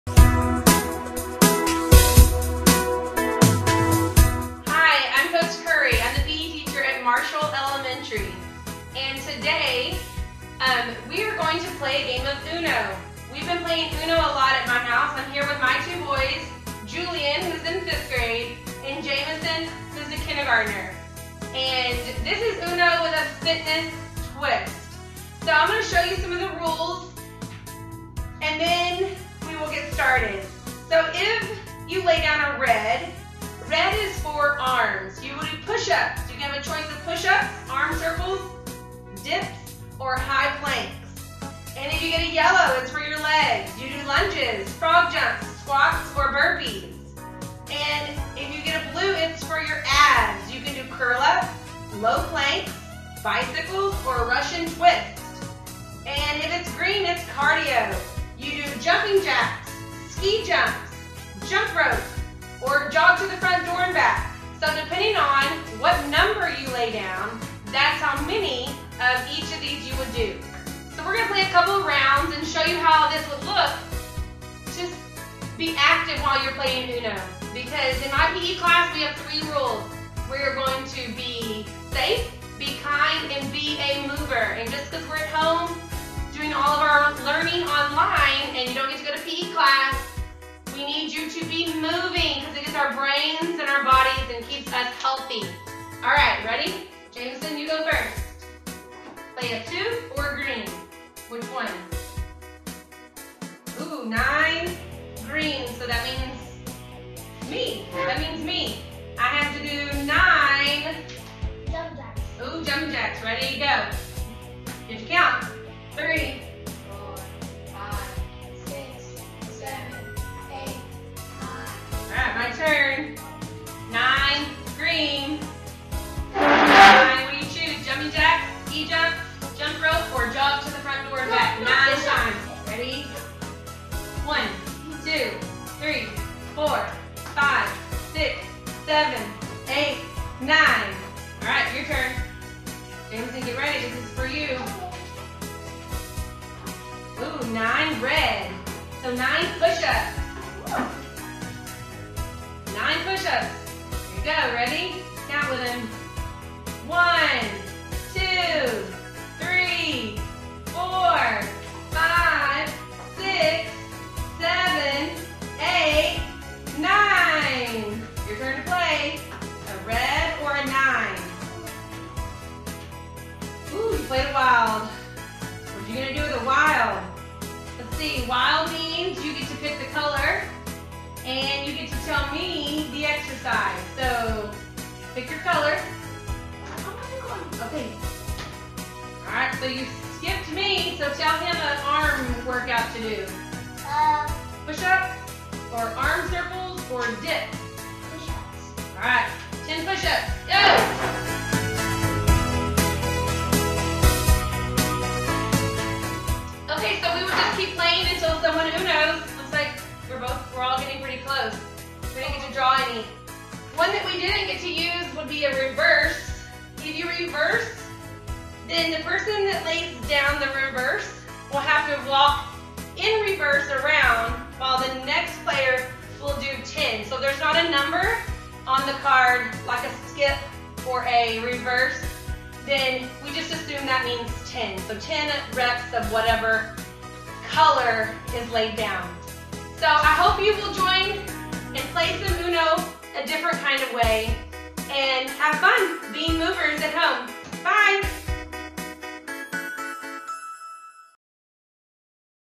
Stay Active Spring - UNO with a FITNESS TWIST!
Our PE Coach at Marshall Elementary, Lacy Curry, is stopping by today for a game of UNO. Actually, it's UNO with a fitness twist!  Thanks to Coach Curry and Marshall ES for helping us stay active and healthy while we're at home!